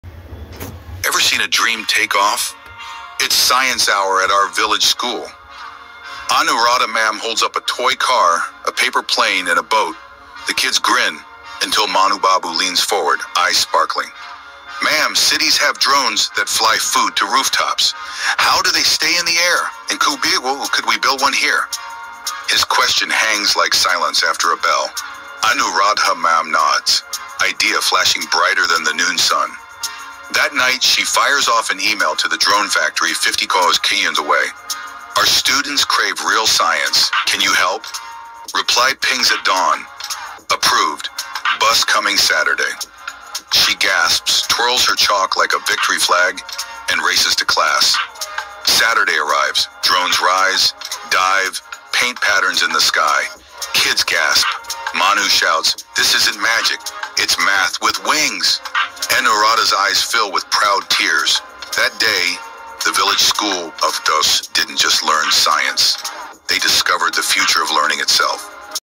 "Rural Kids + Tech = Bright Future 💡📱"
What happens when a 12-year-old village boy asks about drones?
Anuradha Ma’am sparks a revolution in rural learning!
Watch how one small school turns a simple question into a flying dream — with science, wonder, and a glimpse of the future.
This is not just a lesson — it’s the beginning of tech-powered transformation for India’s villages.